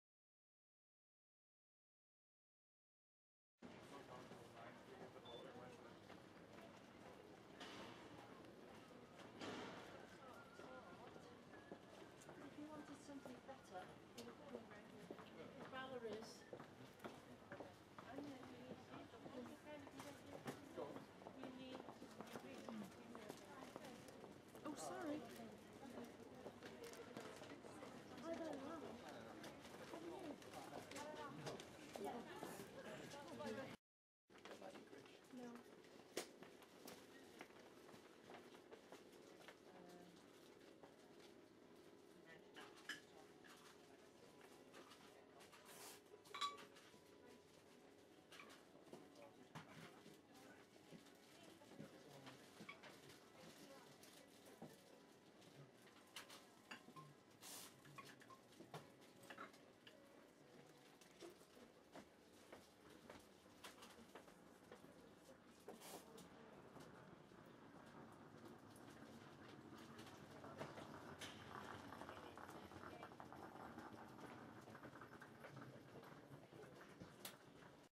The Old Sweet Shop in Chester Town centre
Located opposite the Chester Cathedral it is a Child's (or an Adult's with Child's heart) Paradise